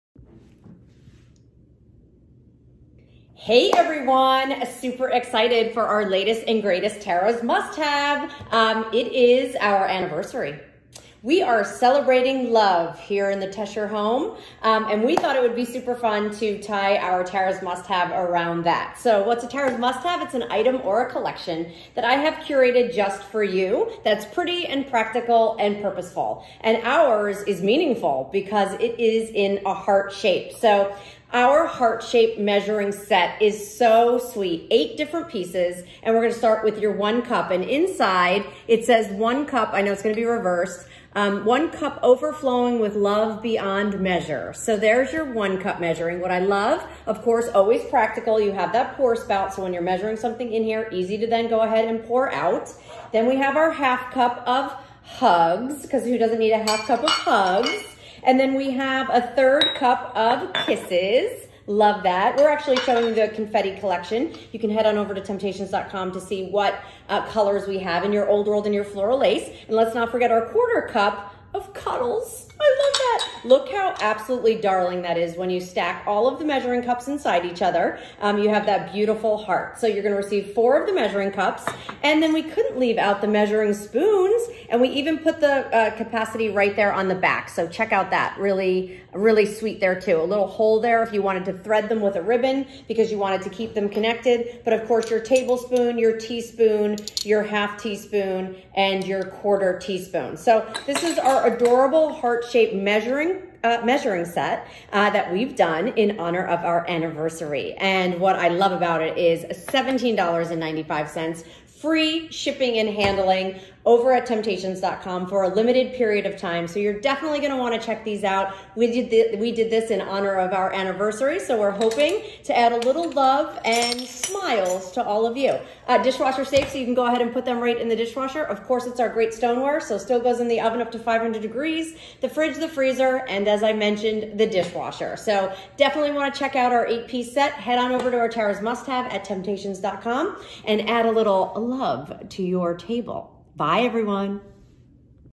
Tara's Must Haves Heart Shaped Measuring Cups & Spoons
💜Pretty
💜Practical
💜Purposeful and full of heart😍
Make your meals with heart, thanks to this set of sweet measuring cups and spoons. Then add extra love and smiles to your kitchen and baking with my latest Tara's Must Haves Heart Shaped Measuring Cups & Spoons.
This item includes:
(1) 1 cup measuring cup
(1) 1/2 cup measuring cup
(1) 1/3 cup measuring cup
(1) 1/4 cup measuring cup
(1) 1 tablespoon measuring spoon
(1) 1 teaspoon measuring spoon
(1) 1/2 teaspoon measuring spoon
(1) 1/4 teaspoon measuring spoon

Dishwasher, microwave, refrigerator, and freezer-safe; oven-safe up to 500°F